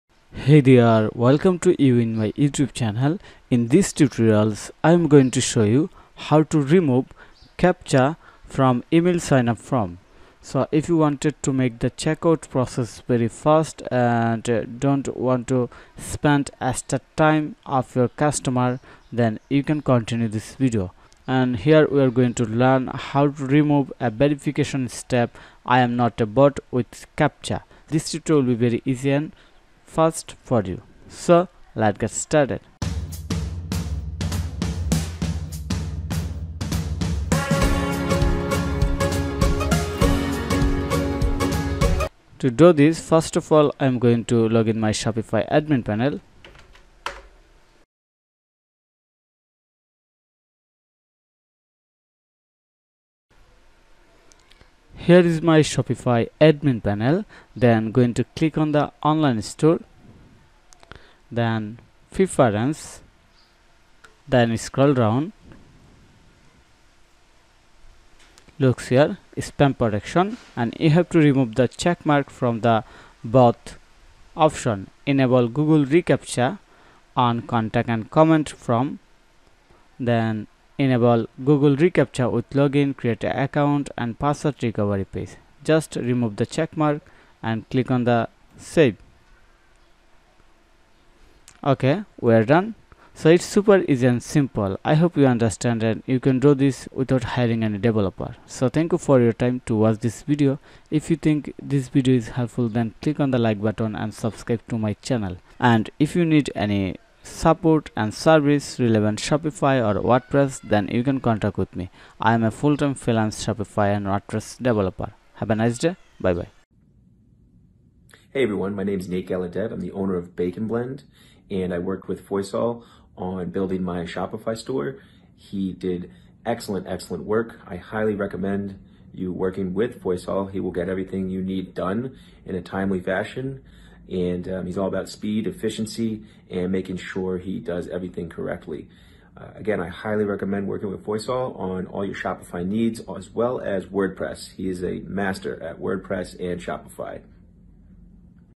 How to remove CAPTCHA from Email Signup Form ✅ Shopify Tutorial for Beginners
💻 Welcome to * How to remove CAPTCHA from Email Signup Form ✅ Shopify Tutorial for Beginners *

📝 Video Description: Ditch the CAPTCHA hassle! Learn how to effortlessly remove it from your Shopify email signup form and boost conversions with this beginner-friendly tutorial.